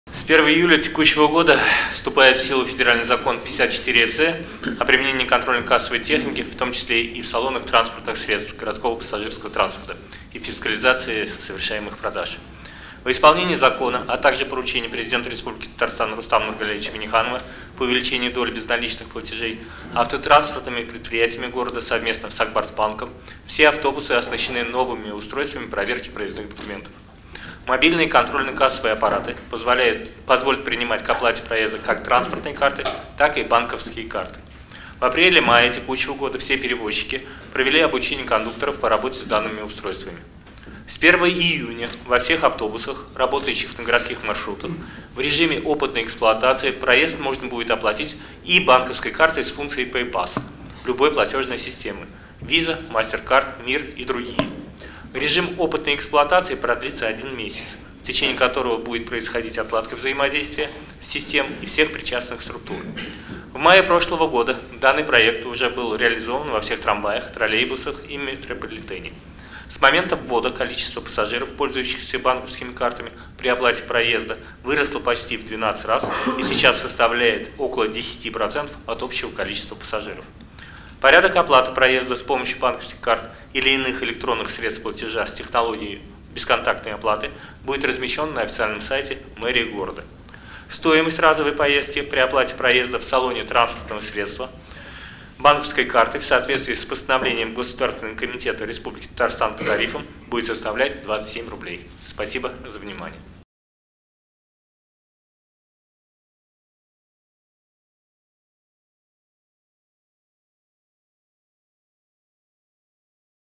С 1 июня во всех автобусах Казани можно будет расплачиваться банковской картой
С 1 июня в режиме опытной эксплуатации во всех казанских автобусах можно будет расплачиваться и транспортной, и банковской картой с функцией PayPass. Партнером проекта выступил Ак Барс Банк. Об этом в ходе утреннего совещания в исполкоме сообщил глава комитета по транспорту Айдар Абдулхаков. Стоимость проезда составит 27 рублей, тестовый режим продлится один месяц.